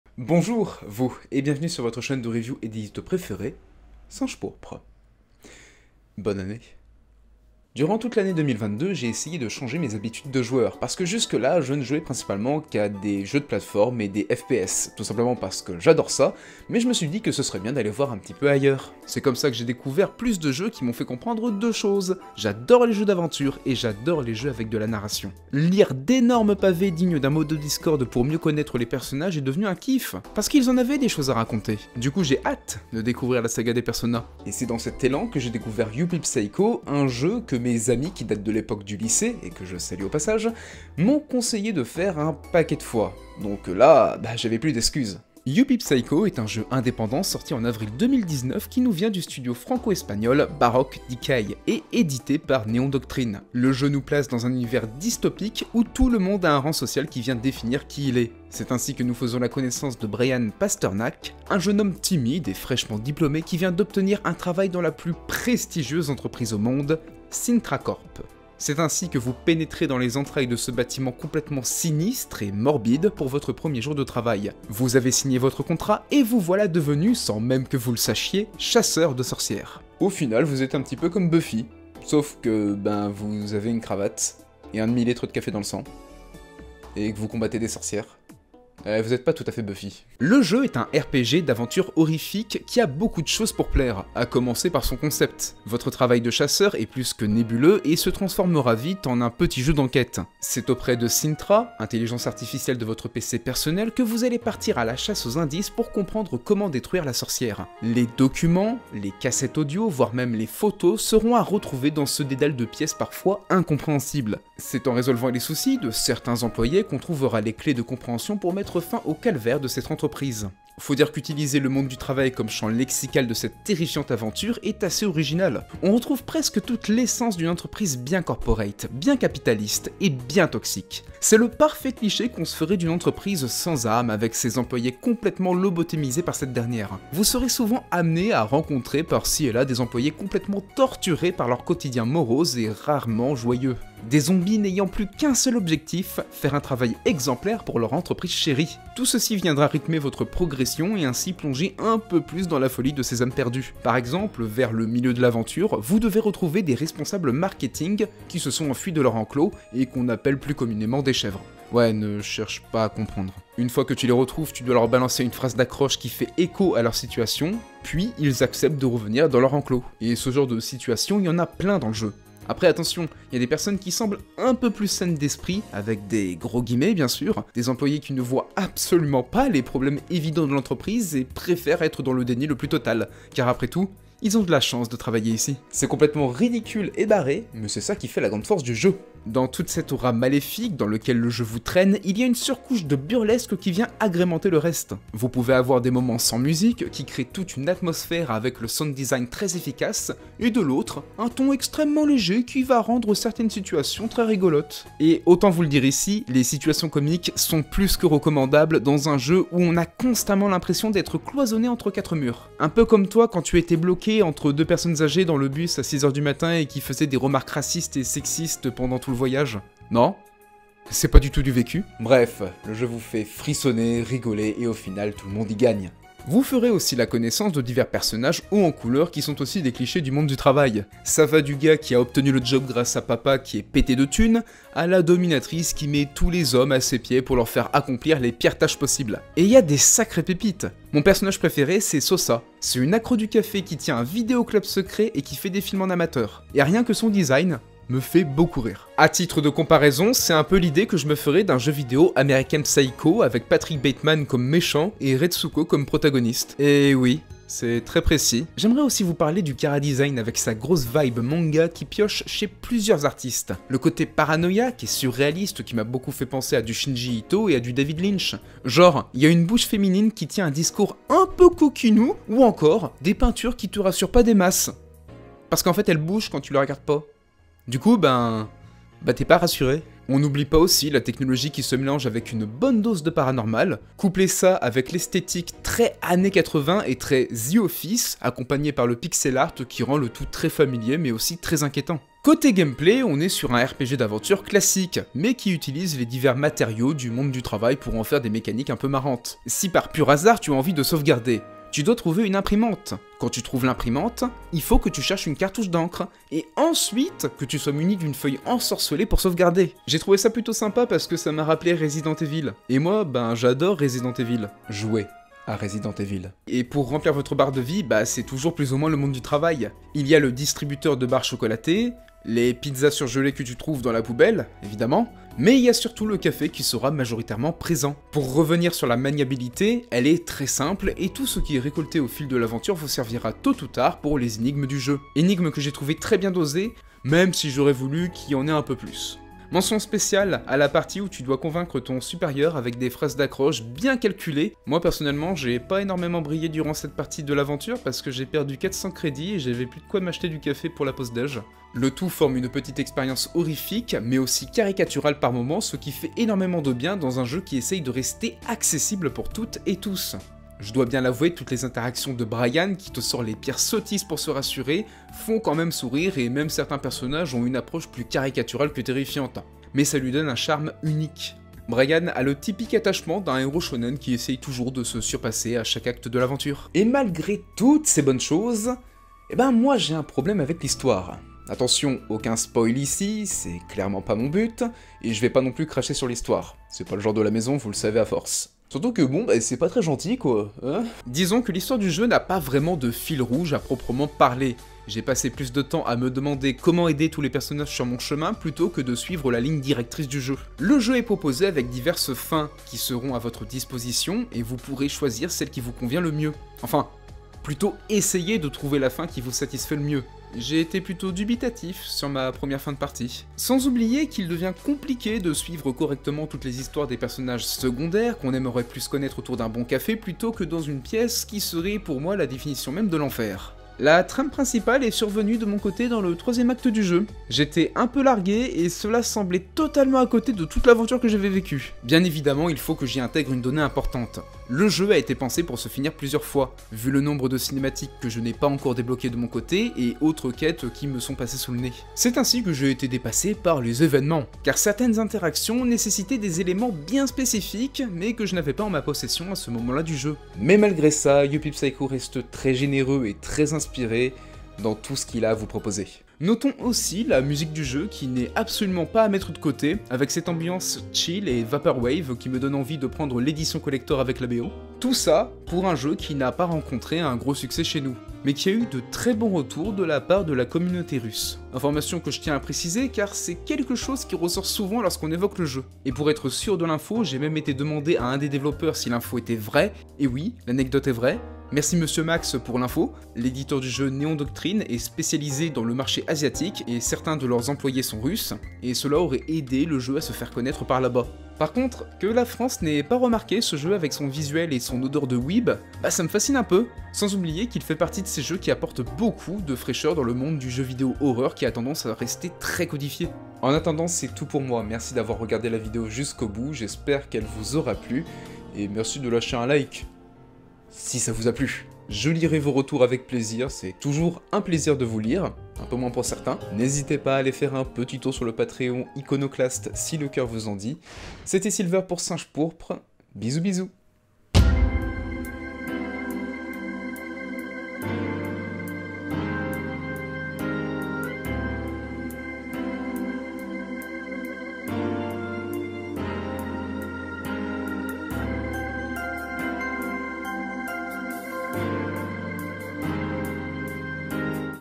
N'accepte jamais un travail au pif à Paris | Yuppie Psycho
Bonjour... vous! Cette semaine c'est Silver qui vous parle de Yuppie Psycho, un jeu d'horreur en pixel art qui sent bon le désordre mental. Attention, âme sensibles s'abstenir.